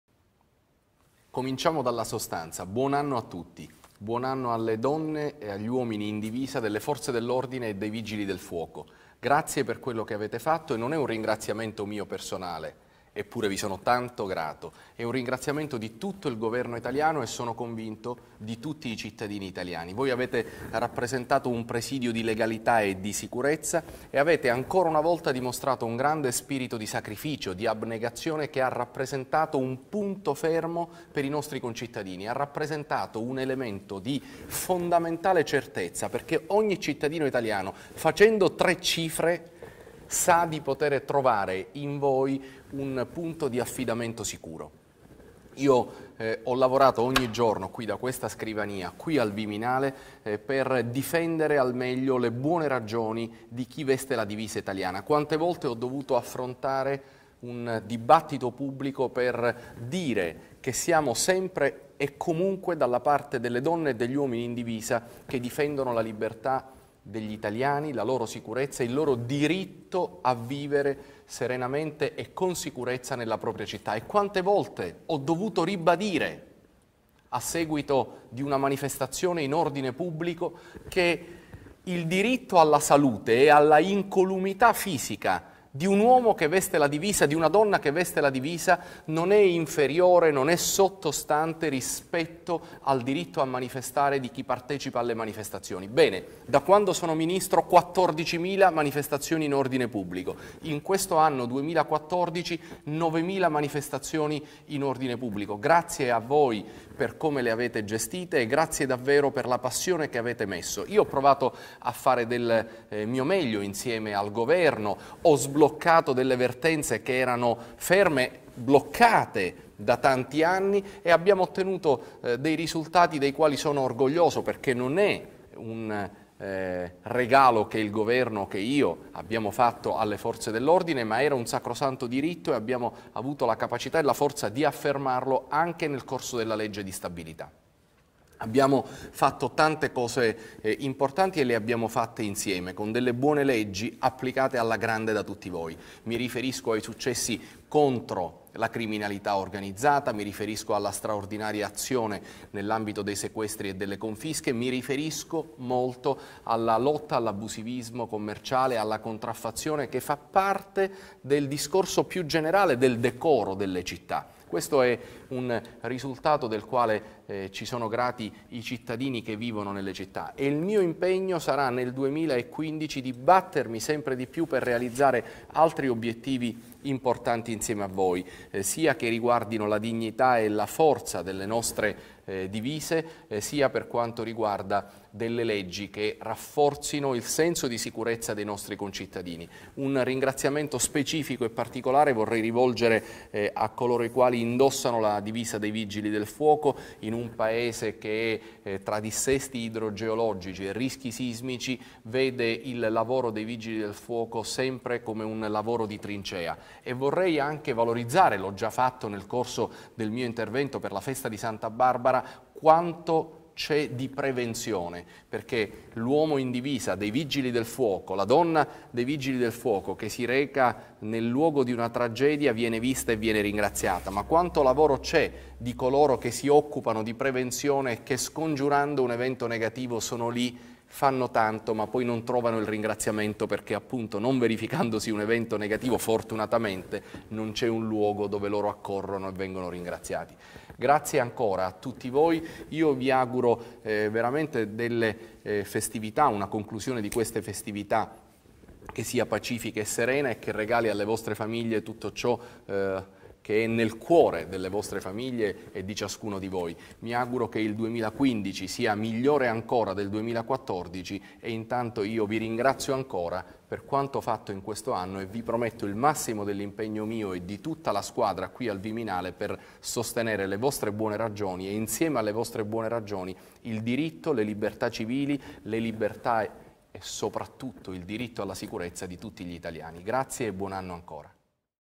Auguri di fine anno del ministro Alfano
Come da tradizione, il ministro dell’Interno chiude l’anno con un discorso augurale e un ringraziamento alle Forze dell’ordine per il lavoro svolto. Notizia